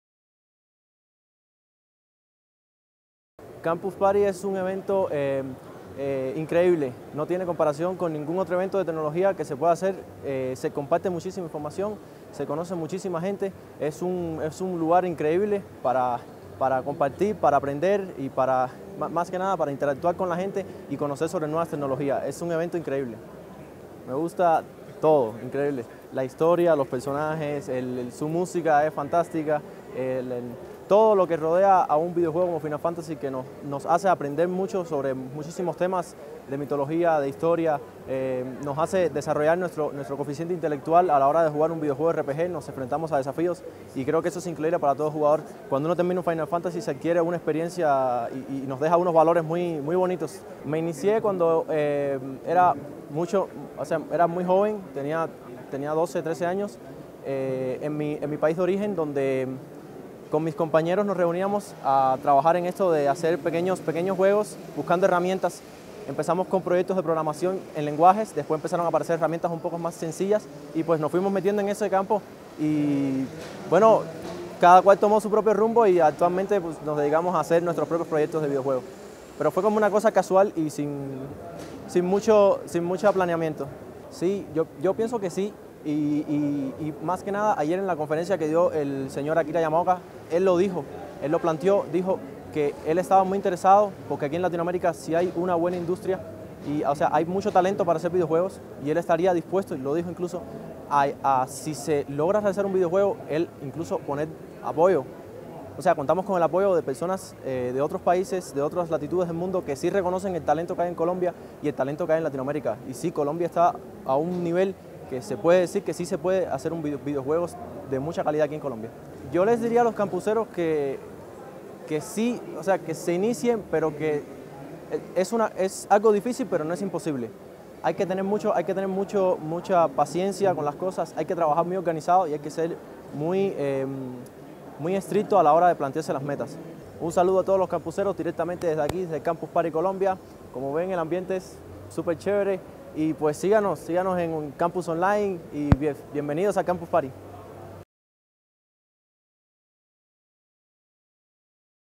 CPCO5 - Entrevista Emilio Rodríguez
Emilio Rodríguez Estudiante de Ing. de Sistemas de la Universidad de Cuenca, en Ecuador. Aprendiz autónomo con siete años de experiencia de tutor y promotor de RPG Maker, es miembro del Microsoft Student Tech Club "Bluecell 2.0" a nivel universitario y miembro élite desarrollador de Final Fantasy Community. Así mismo, es creador y difusor de desarrollo de proyectos independientes, webstores y sitios web.